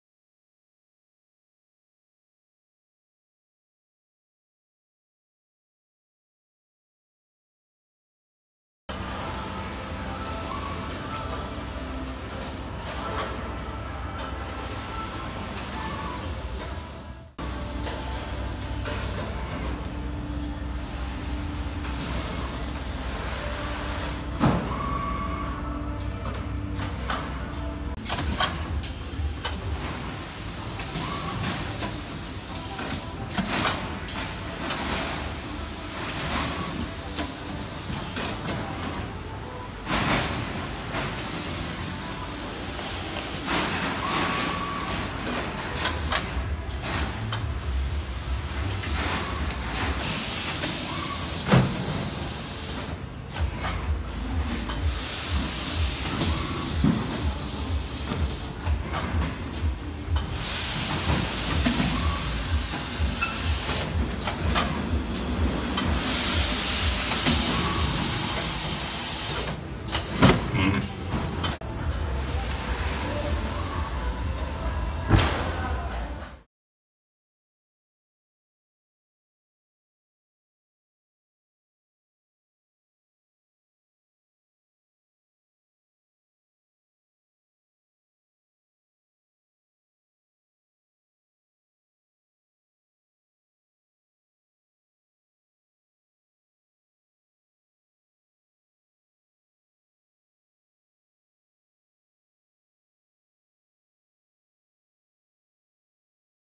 Bercomex ContainerFill - Bucket filling system
Automatic de-stacking and filling of barrels? The ContainerFill can do it. How fast? Up to 900 buckets per hour, depending on liters and water pressure. The ContainerFill is suitable for square auction containers. With this machine, containers are always ready to be filled with flowers.